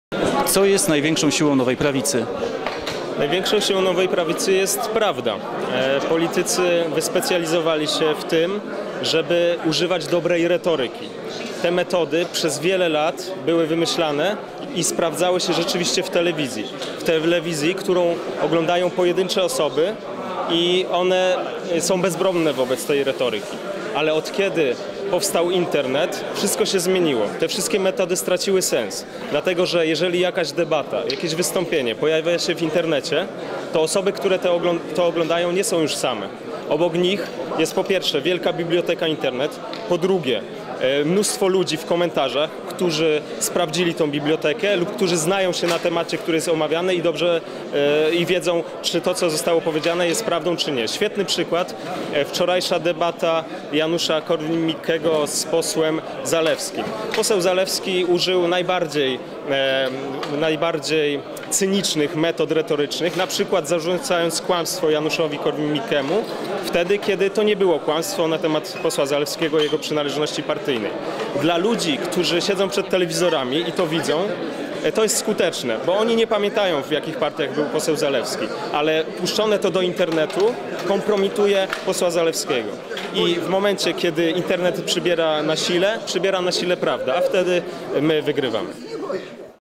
Konrad Berkowicz Nowa Prawica Janusza Korwin-Mikke - Konwencja Wyborcza
Konwencja Wyborcza Kongresu Nowej Prawicy Janusza Korwin-Mikke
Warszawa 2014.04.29